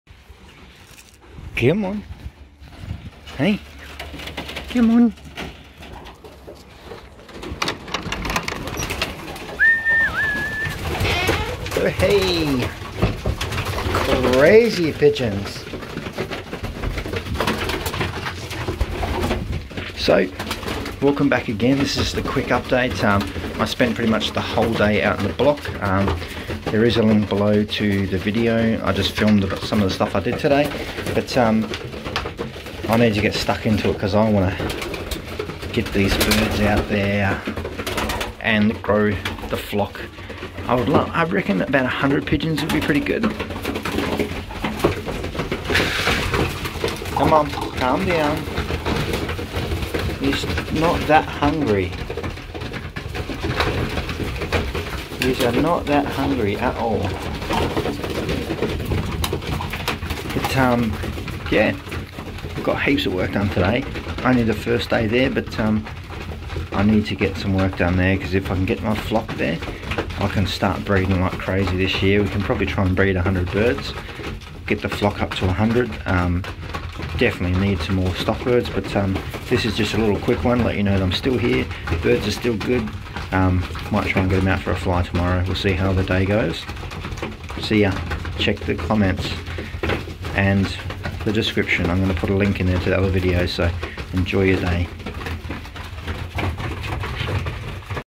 Pigeon Update 😀
Only a very short one, the last 2 days have been so busy! i will aim to get a flock down series up tomorrow at some point.